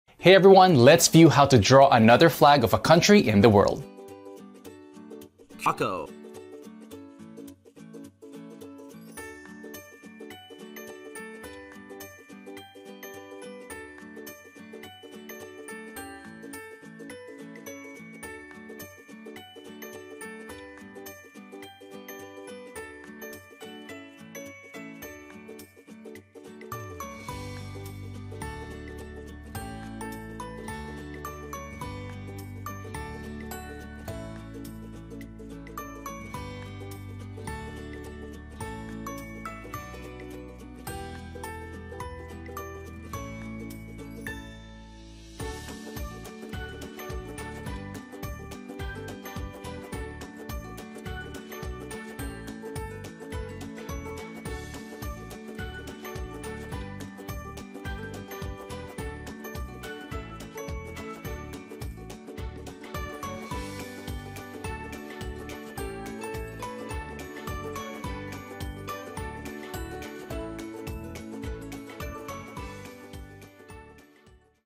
Flag of Curacao - 🌍 How to Draw the Curacao Flag - SVG Vector
- Flag of Curacao: Check out how to draw the flag of Curacao. Let’s discover how to draw the national flag of Curacao in a few minutes. Discover the national flags of all the countries in the world.